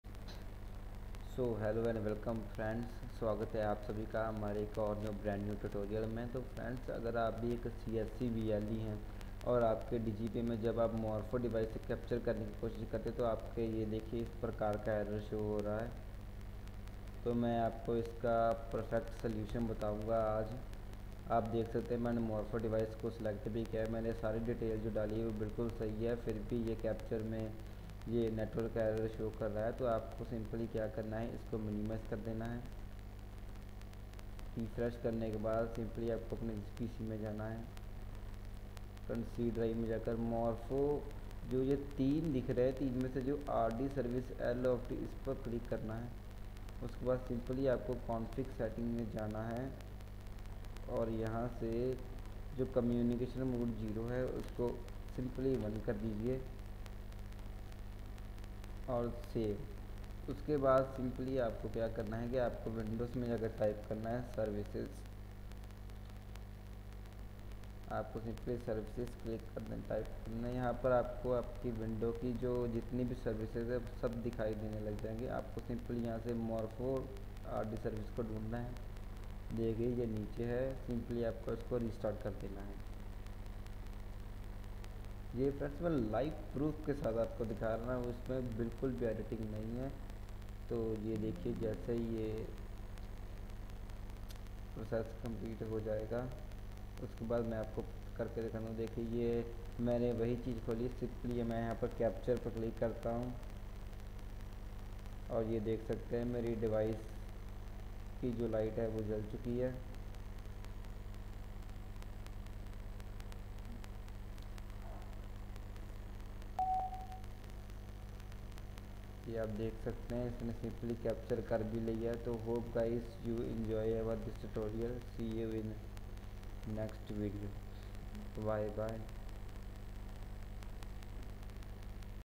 Digipay lite morpho device error solve
today in this video i show you how to fix digipay lite error